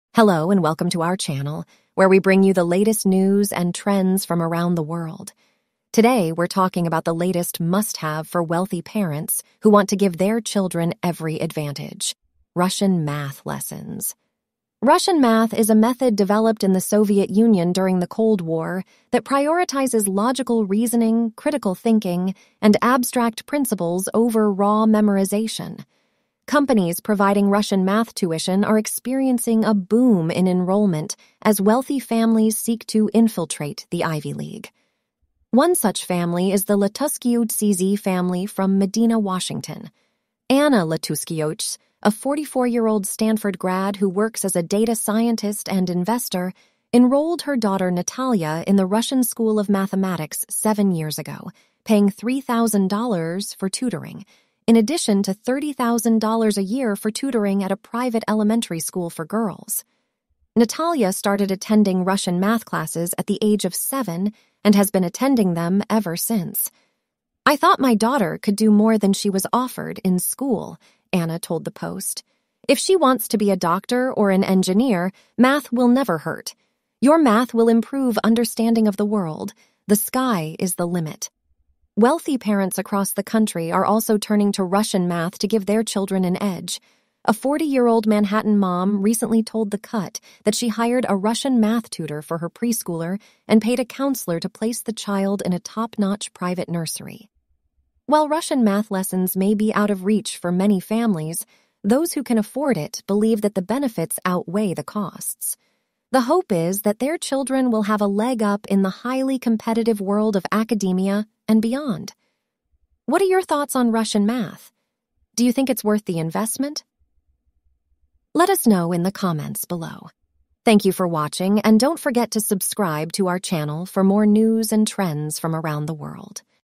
How Russian Math Became a Must Have Extracurricular for Overachieving Parents
Wealthy parents are enrolling their children in Russian math lessons to give them an edge in logical reasoning, critical thinking, and abstract principles over raw memorization. The method, developed in the Soviet Union during the Cold War, is experiencing a boom in enrollment as families seek to infiltrate the Ivy League. Companies providing the tuition are seeing an increase in demand for their services as parents aim to give their children every advantage.